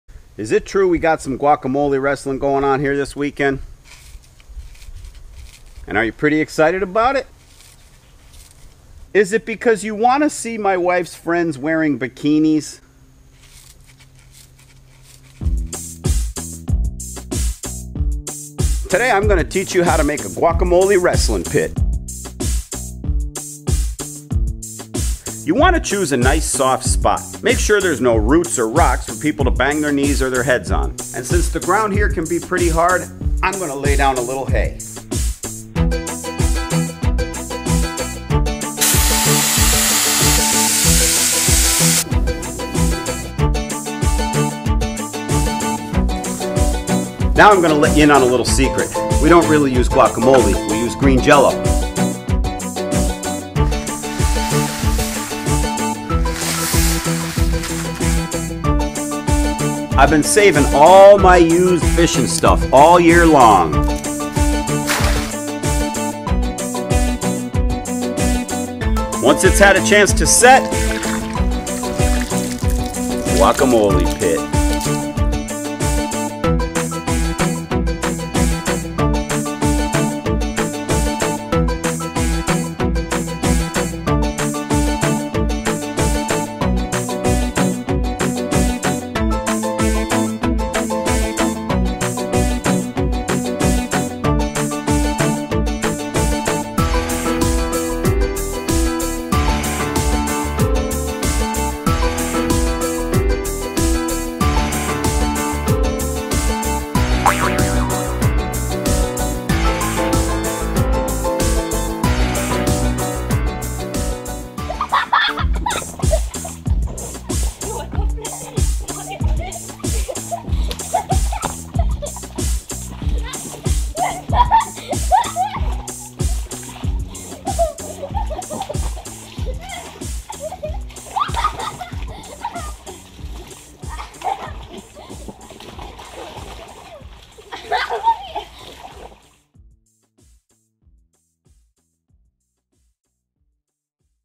Sleepy Lizard Guacamole Wrestling Pit
Here's how we make a fun guacamole wrestling pit for our annual halloween bash.